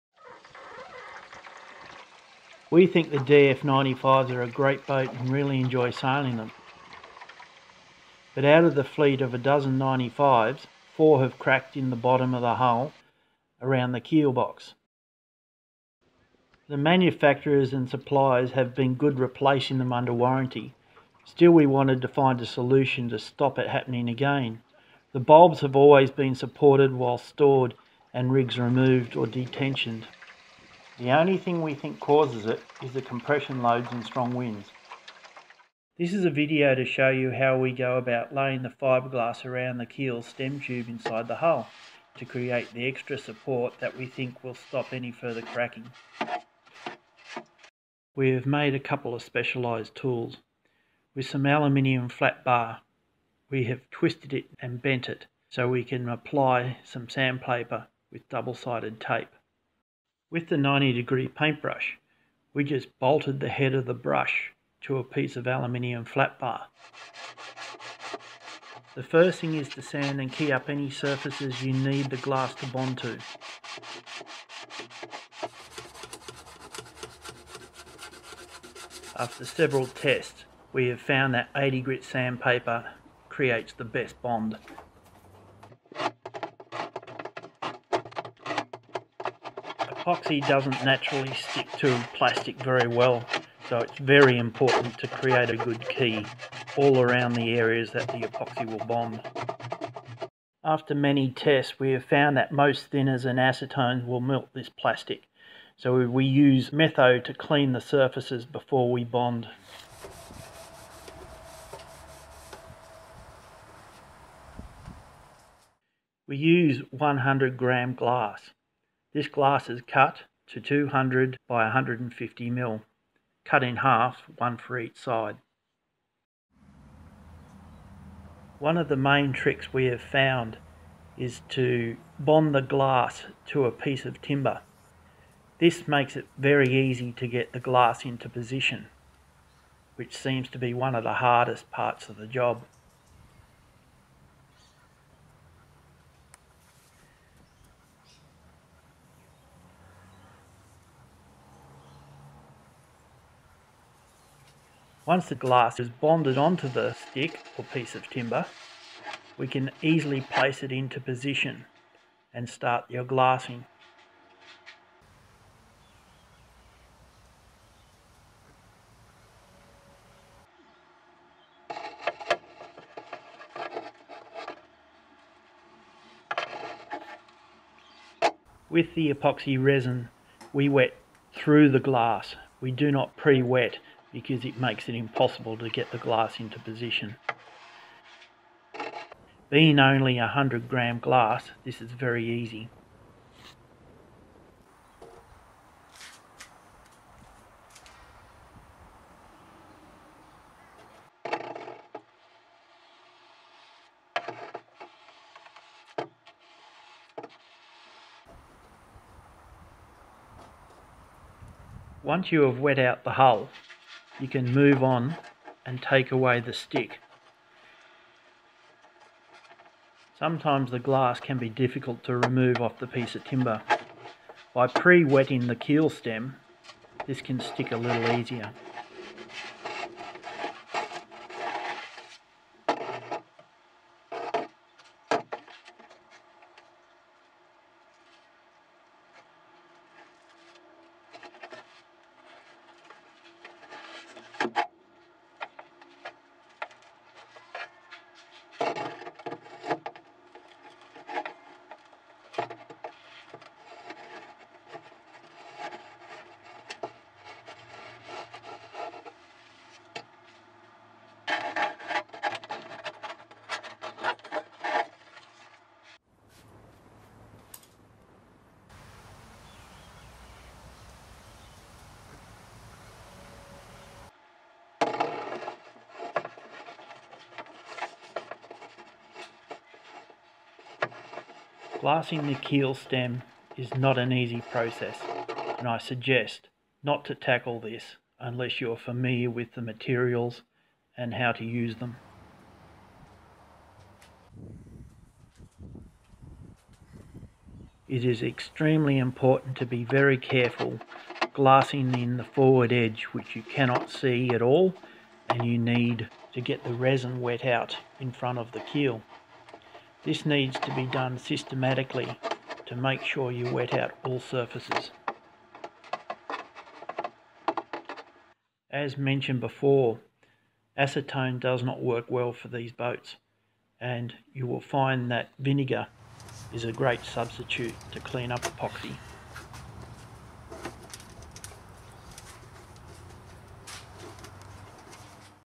Dragonflite 95 Hull Cracks Fibreglass Reinforcement
Some of the Whitsunday Model Boat Club members have had issues with cracks forming in the hulls of their DF95s and we have come up with a method to strengthen the plastic hull with fibreglass around the keel stem.